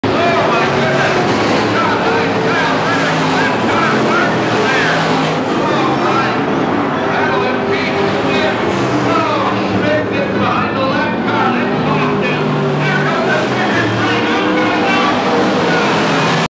4.4.15 Seymour Spring Thaw - IMCA Stock car final lap
57 John Heinz last lap battle in IMCA Stock with 50F Kyle Frederick at the first races of 2015 in Wisconsin.